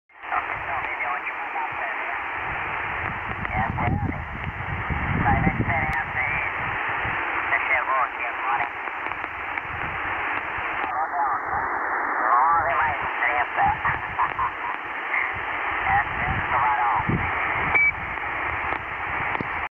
27.775 Khz CB 10 JUL 2022. Hr 22:19 (20:19 UTC) Teruel, España /Spain.
27.775 Khz USB extranjeros en CB.
Recibido con Tecsun PL330 y su antena telescópica.
Recibido en el norte de la provincia de Teruel, España.

27.775 Khz USB foreigners on CB.
Received with tecsun PL330 and his telescopic antenna.
Received at north of Teruel province. Spain.